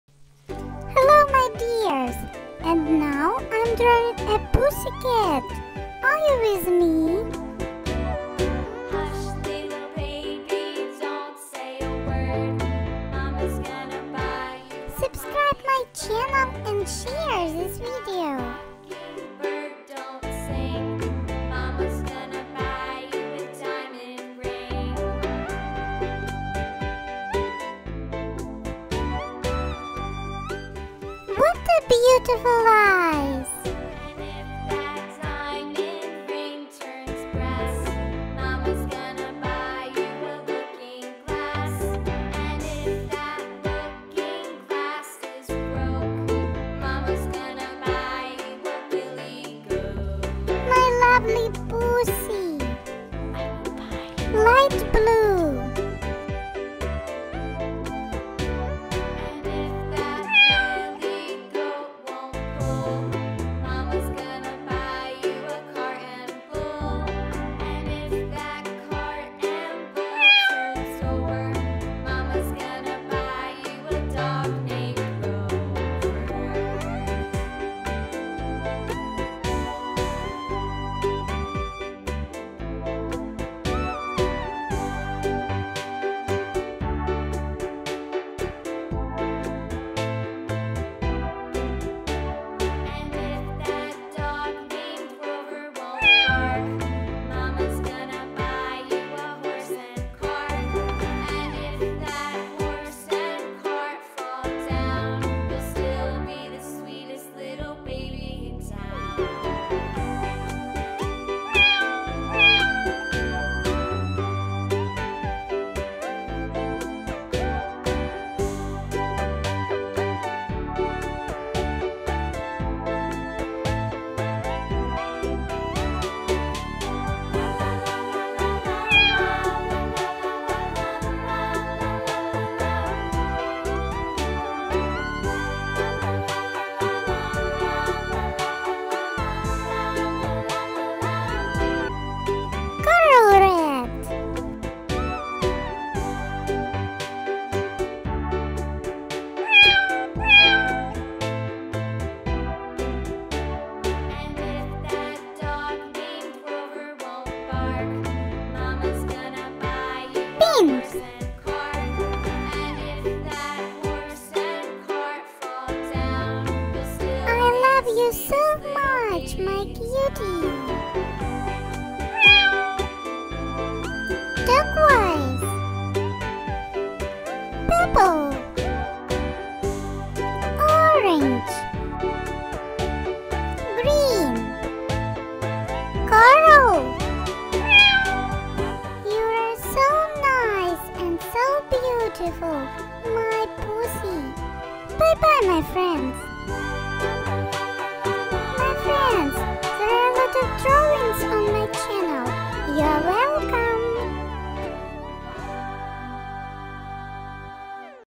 Pussy Cat Coloring,Drawing and Painting for Kids, Toddlers
Hello everyone!
I"ll draw a nice pussy .are you with me?Let"s draw and learn coloring, drawing and painting.